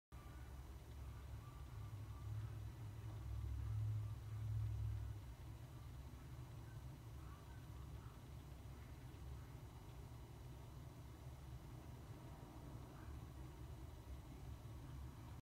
3D Print Clocks by GO-3D Print
Here is our Round Desktop clock, fully 3D printed and what you need is a clock movement kit that cost $2-$4 in Amazon or eBay. You can custom or build your own clock arms.

Clock Movement Kits:

Download and print go3d round